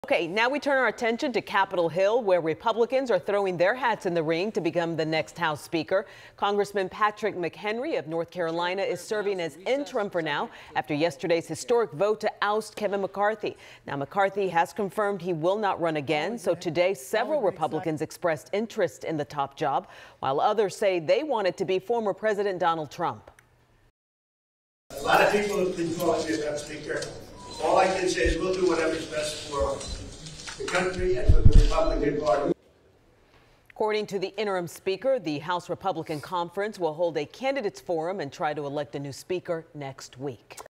Several Republicans vying for House speaker after McCarthy ousted
A candidates forum is scheduled to be held soon.